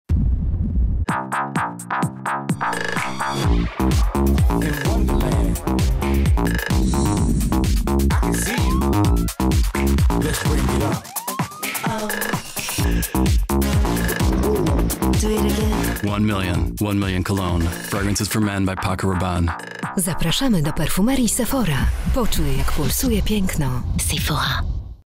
Sephora prezentuje: Paco Rabanne 1 Million Cologne
Świeżość warta milion dolarów rozpisana na aromaty,
które skruszą lód. Nowa przygoda odkrywa przed współczesnym mężczyzną akord morski połączony z koktajlem owoców cytrusowych i kardamonem, które wtórują niebywale męskiemu tchnieniu korzennej skóry, ożywionej aromatem bobu tonka.